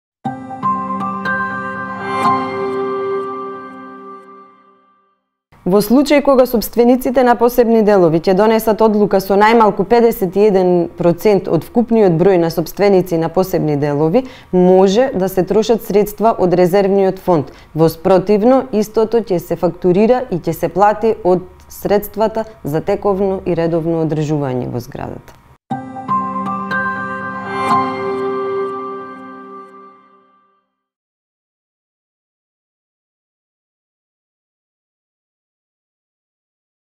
Што да направиме за да се плати од резервниот фонд за чистење на зградата?
Одговара Јасмина Петреска од Регулаторната комисија за домување.